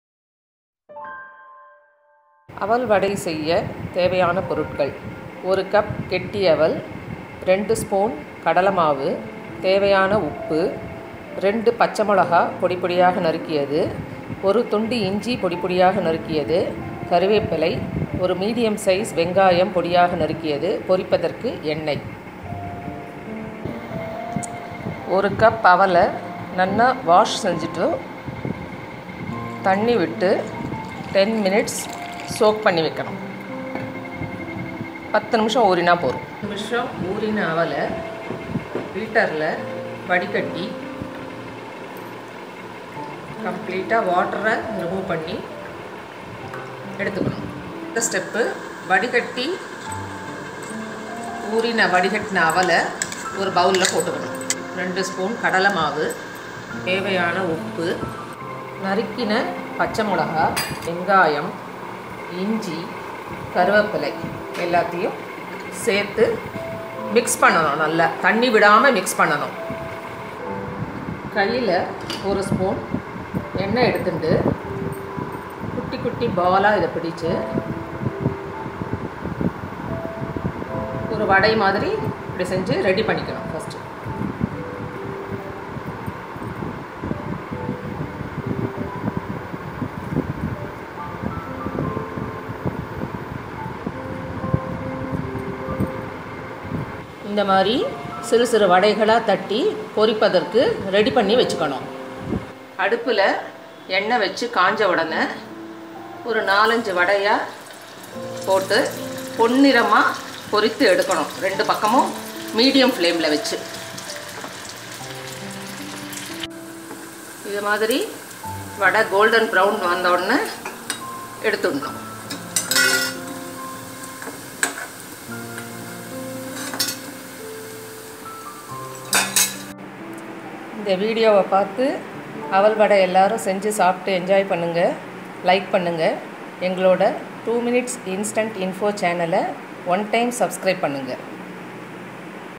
Aval Vadai /Poha Vada / Flattened Rice deep fry Dumplings
This video is about 2 Mins Instant Recipe: Aval Vadai Poha Vada Beated-Rice instant tiffin for Guest
Very quick recipe and we always enjoy whenever we make this at home.